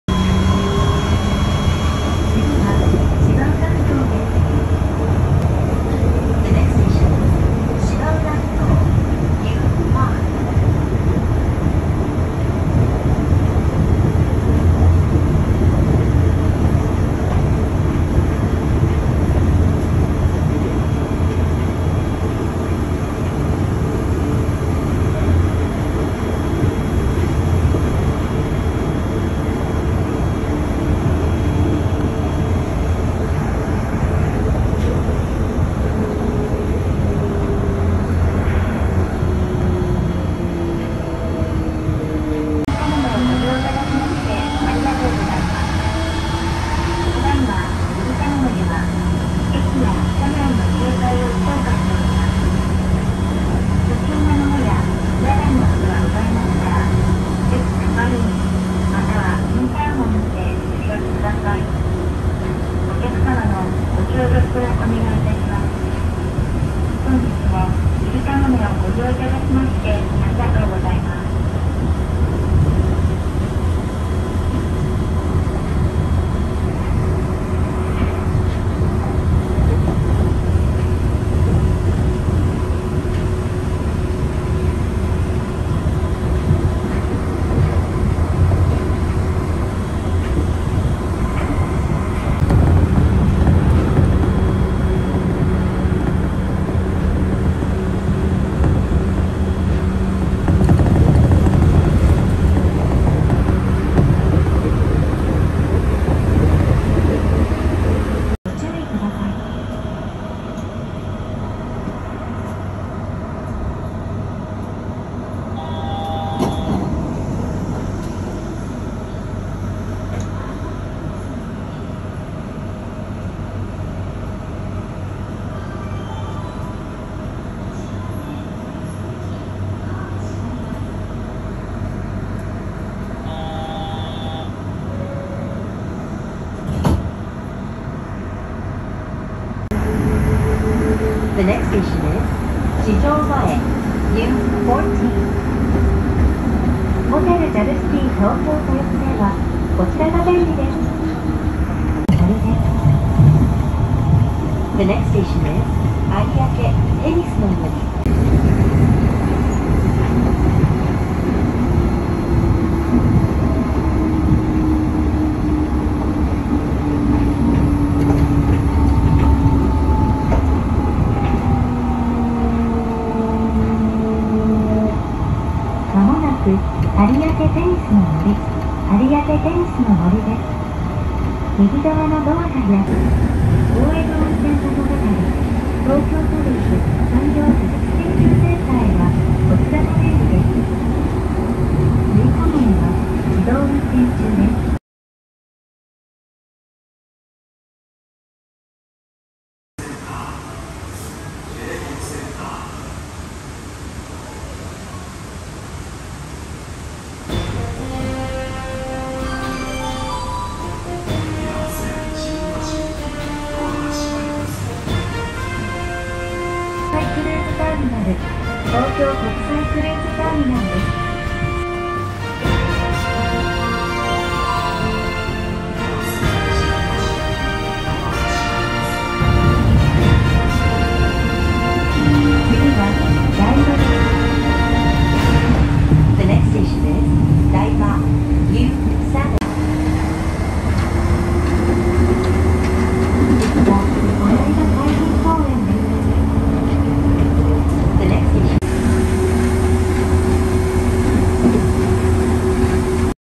Olympic site tour
Quick trip around the Tokyo Olympic sites
Olympic village and the Broadcast centre